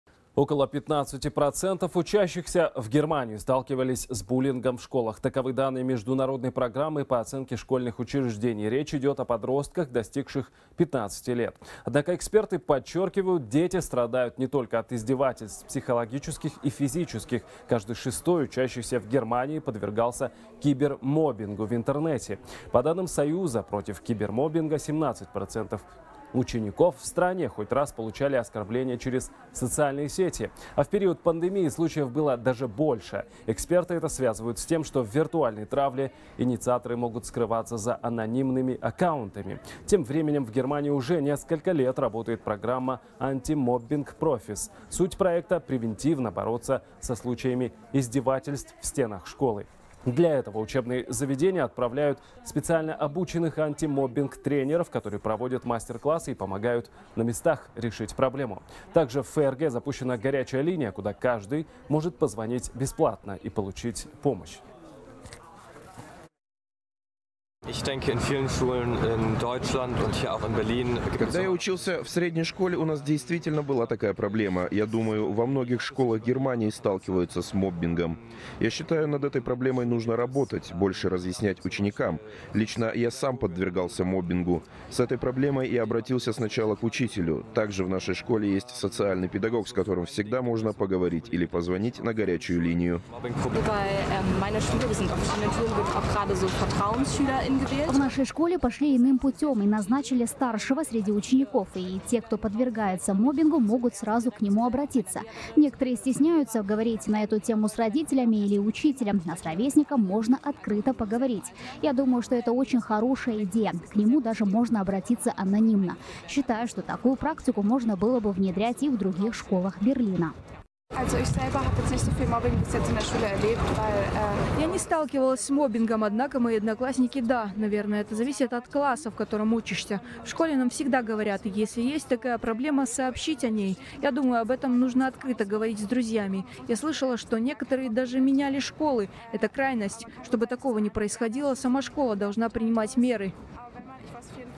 15% учащихся в Германии сталкивались с буллингом в школах
Около 15% учащихся в Германии сталкивались с буллингом в школах, сообщает корреспондент «Хабар 24». Таковы данные международной программы по оценке школьных учреждений.